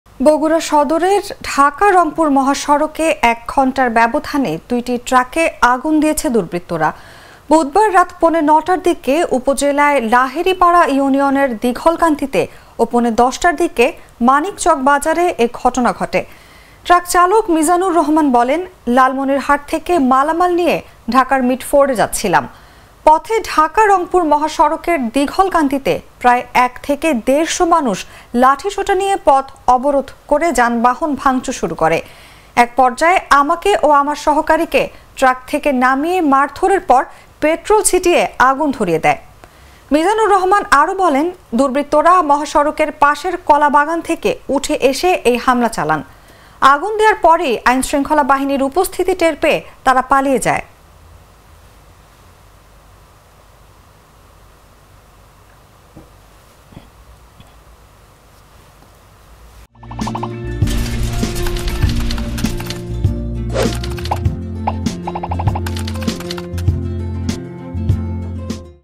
বগুড়ায় এক ঘণ্টার ব্যবধানে ২ ট্রাকে আ গু ন || Jago News
বগুড়ায় এক ঘণ্টার ব্যবধানে ২ ট্রাকে আ গু ন